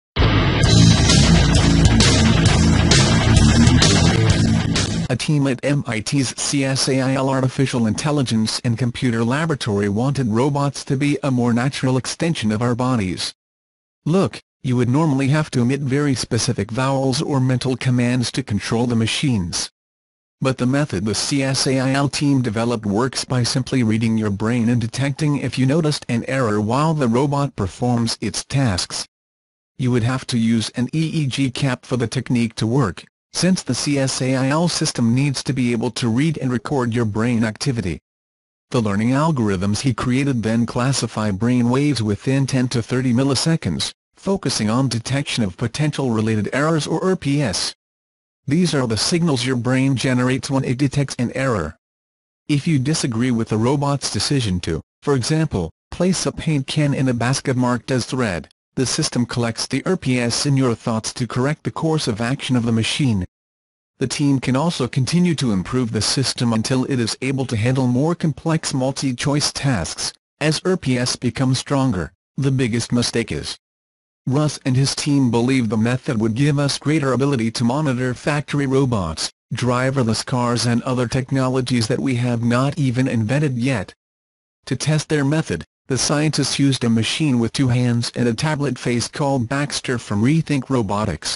MIT: Find an easy way to control robots with your brain.
A team at MIT's CSAIL Artificial Intelligence and Computer Laboratory wanted robots to be a more natural extension of our bodies. Look, you would normally have to emit very specific vowels or mental commands to control the machines. But the method the CSAIL team developed works by simply reading your brain and detecting if you noticed an error while the robot performs its tasks.

You would have to use an EEG cap for the technique to work, since the CSAIL system needs to be able to read and record your brain activity. The learning algorithms he created then classify brain waves within 10 to 30 milliseconds, focusing on detection of potential related errors or ErrPs. These are the signals your brain generates when it detects an error. If you disagree with a robot's decision to, for example, place a paint can in a basket marked as thread, the system collects the ErrPs in your thoughts to correct the course of action of the machine.

The team can also continue to improve the system until it is able to handle more complex multi-choice tasks, as ErrPs become stronger, the biggest mistake is. Rus and his team believe the method would give us greater ability to monitor factory robots, driverless cars and other technologies that we have not even invented yet. To test their method, the scientists used a machine with two hands and a tablet face called Baxter from Rethink Robotics.